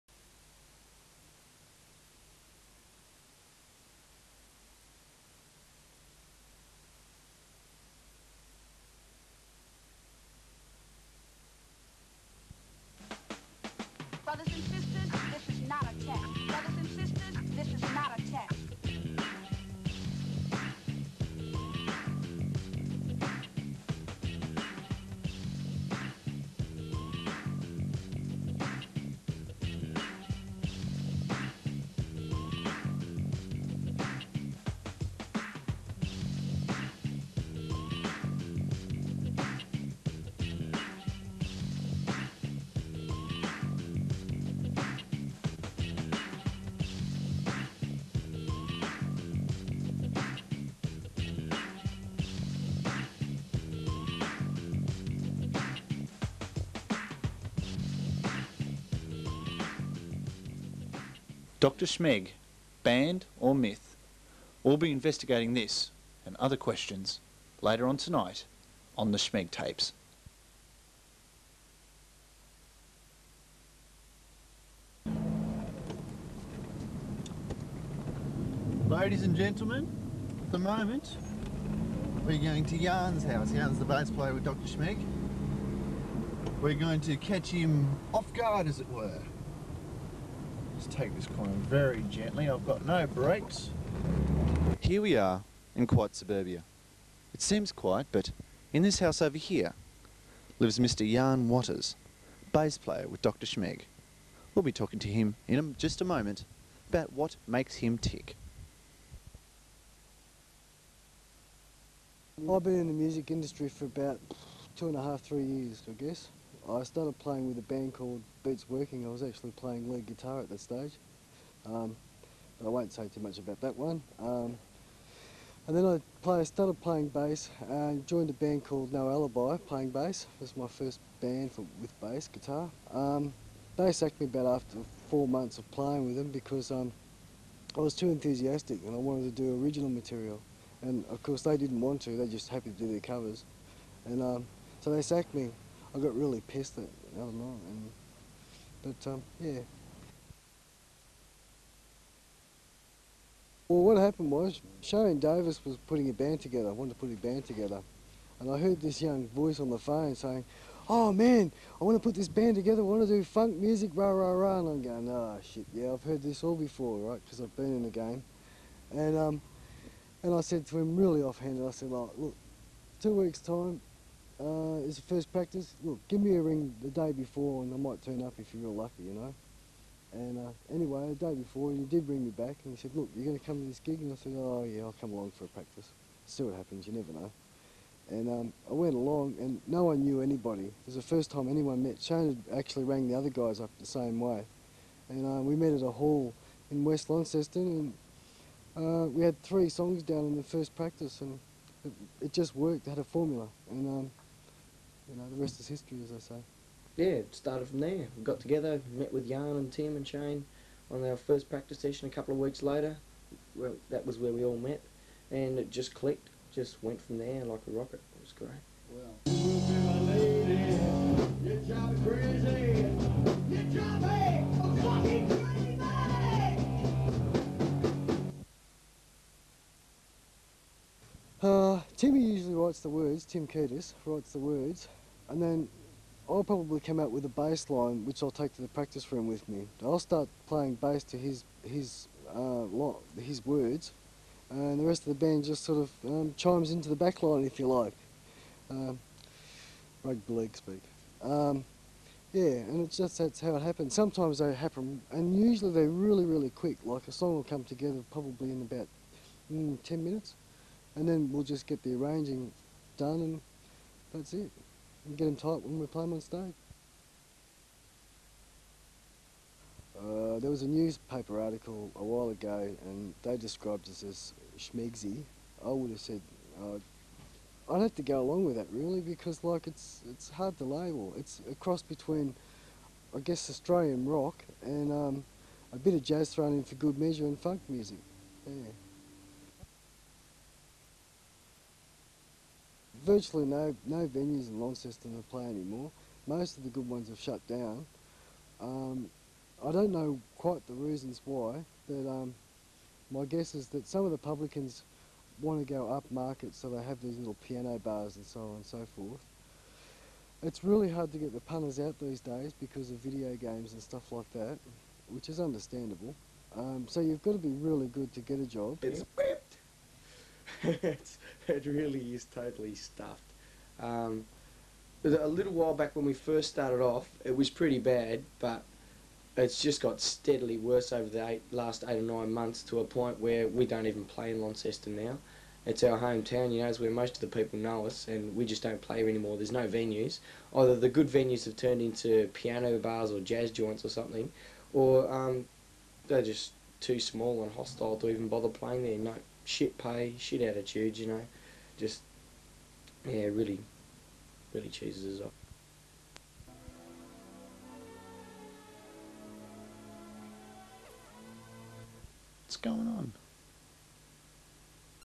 The Schmeg Tapes - Part 1 - The Miracle of Jan
In 1994, 2 sound engineering students made the definitive documentary about the legendary local Launceston band "Doctor Schmeg"